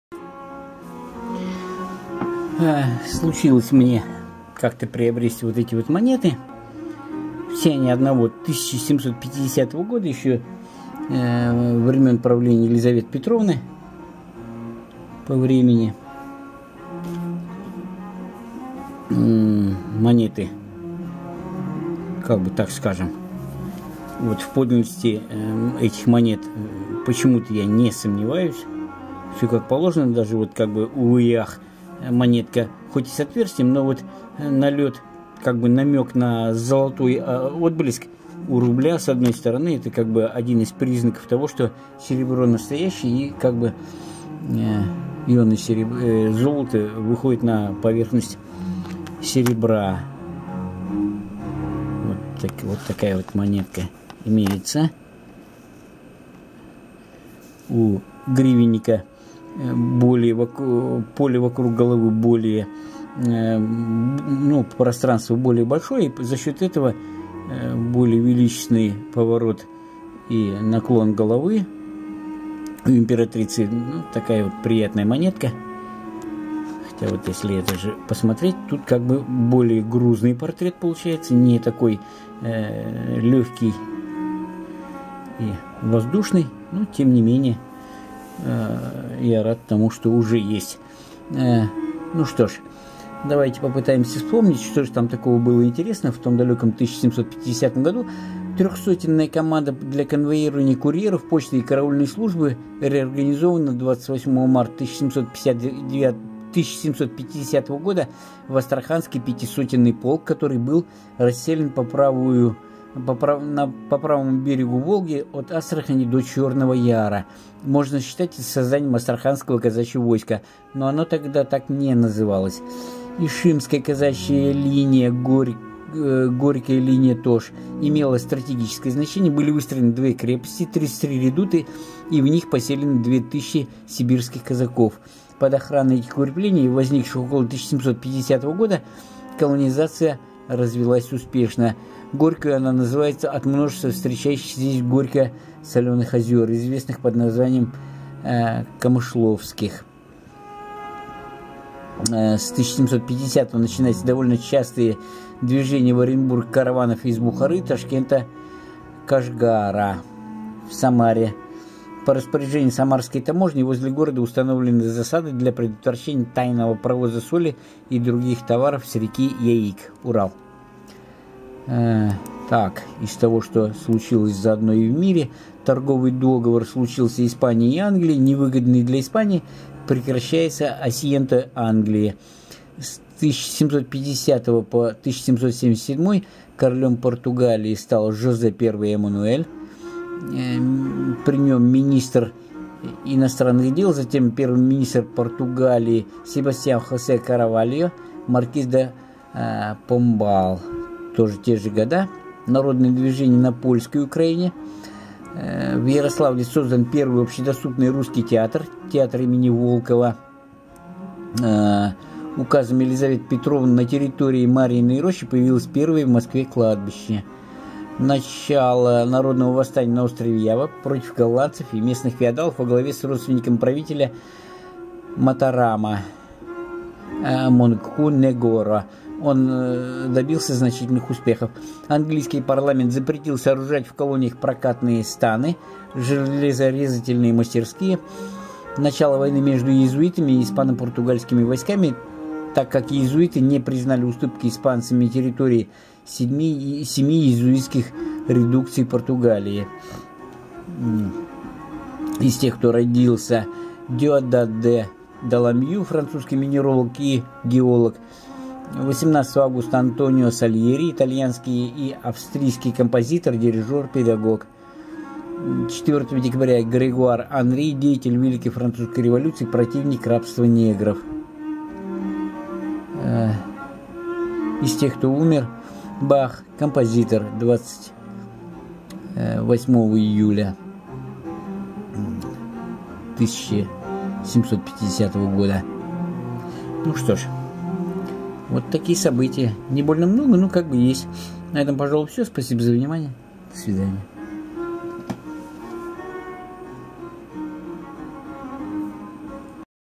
События истории России и мира за 1750 год
1750 (тысяча семьсот пятидесятый) год по григорианскому календарю — невисокосный год, начинающийся в четверг. Это 1750 год нашей эры, 750 год 2 тысячелетия, 50 год XVIII века, 10 год 5-го десятилетия XVIII века, 1 год 1750-х годов.